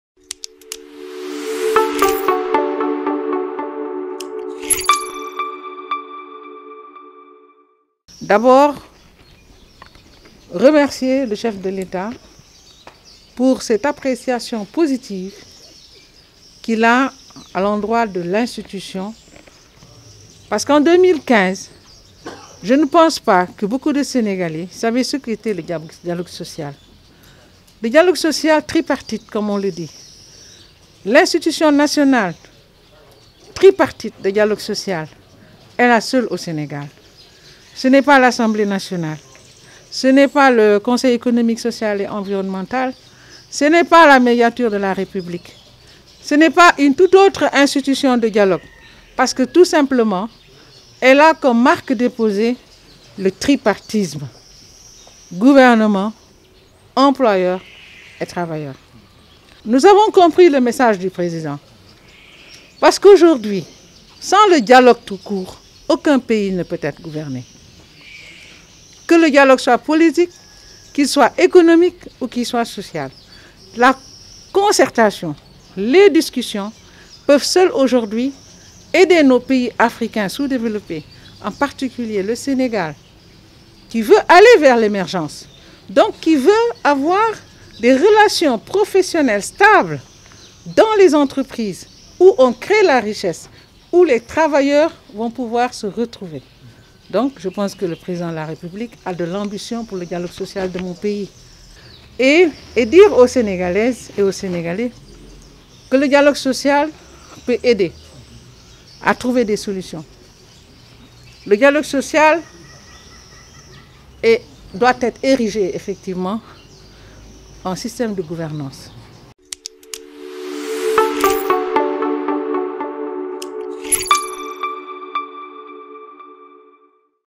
DIALOGUE SOCIAL COMME OUTIL DE GOUVERNANCE DES SECTEURS SOCIAUX.INNOCENCE FELICITE LE CHEF DE L'ETAT
DIALOGUE SOCIAL COMME OUTIL DE GOUVERNANCE DES SECTEURS SOCIAUX.INNOCENCE NTAP NDIAYE FELICITE LE CHEF DE L'ETAT